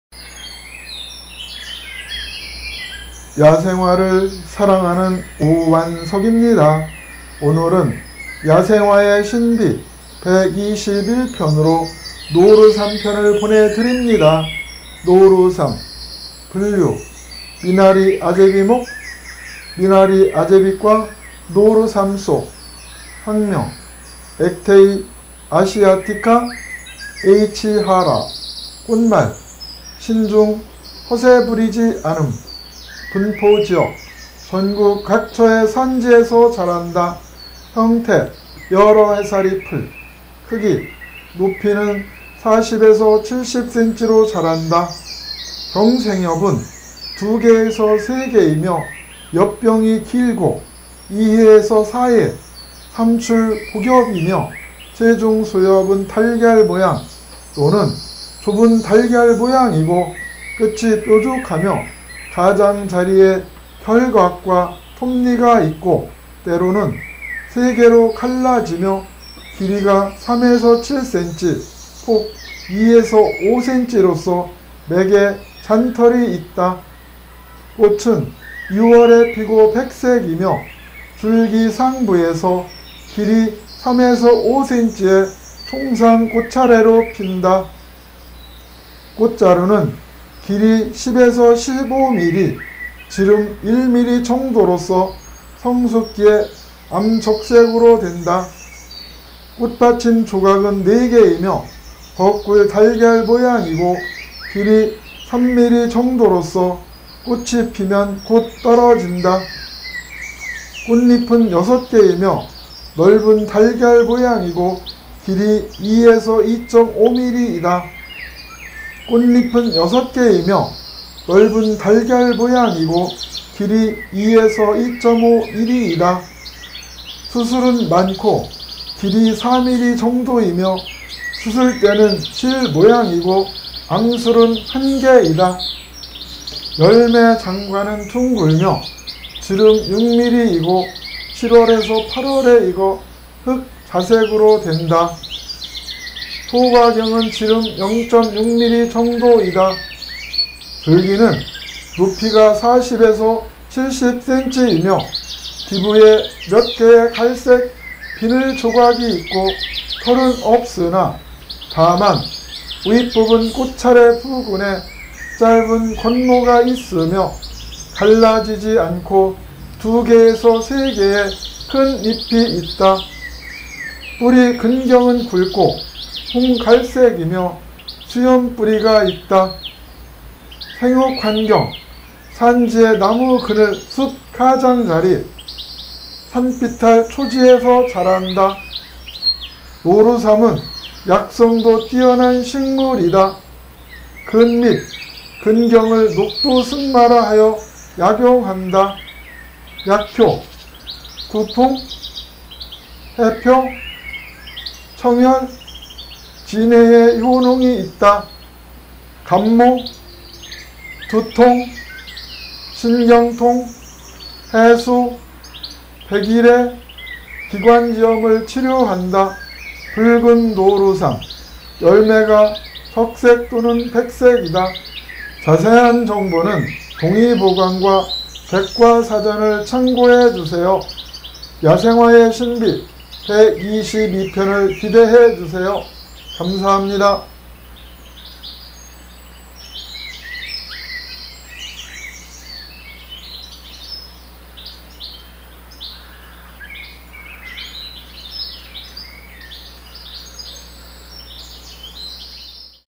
노루삼, 야생화의신비 121편
노루삼
분류: 미나리아재비목, 미나리아재비과, 노루삼속
학명: Actaea asiatica H. Hara
꽃말: 신중, 허세부리지 않음
분포지역: 전국 각처의 산지에서 자란다.
형태: 여러해살이풀
크기: 높이는 40~70cm로 자란다.

경생옆은 2~3개이며 엽병이 길고 2~4회 3출복엽이며 최종소엽은 달걀모양
또는 좁은 달걀모양이고 끝이 뾰족하며 가장자리에 결각과 톱니가 있고
때로는 3개로 갈라지며 길이 3~7cm, 폭 2~5cm로서 맥에 잔털이 있다.
꽃은 6월에 피고 백색이며 줄기 상부에서 길이 3~5cm의 총상꽃차례로 핀다.
꽃자루는 길이 10~15mm, 지름 1mm정도로서 성숙기에 암적색으로 된다.
꽃받침조각은 4개이며 거꿀달걀모양이고 길이 3mm정도로서 꽃이 피면 곧 떨어진다.
꽃잎은 6개이며 넓은 달걀모양이고 길이 2~2.5mm이다.
수술은 많고 길이 4mm정도이며 수술대는 실모양이고, 암술은 1개이다.

열매 장과는 둥글며 지름 6mm이고 7~8월에 익어 흑자색으로 된다.
소과경은 지름 0.6mm정도이다.
줄기는 높이 40~70cm이며 기부에 몇개의 갈색 비늘조각이 있고 털은 없으나
다만 윗부분 꽃차례 부근에 짧은 권모(卷毛)가 있으며
갈라지지 않고 2~3개의 큰 잎이 있다.
뿌리 근경은 굵고 홍갈색이며 수염뿌리가 있다.
생육환경 산지의 나무그늘, 숲가장자리, 산비탈 초지에서 자란다.

노루삼은 약성도 뛰어난 식물이다.
根(근) 및 根莖(근경)을 綠豆升麻(녹두승마)라 하며 약용한다.
약효 : 驅風(구풍), 解表(해표), 淸熱(청열), 鎭咳(진해)의 효능이 있다.
感冒(감모), 두통, 신경통, 咳嗽(해수), 백일해, 기관지염을 치료한다.
붉은노루삼 : 열매가 적색 또는 백색이다.
자세한정보는 동의보감과 백과사전을 참고하세요,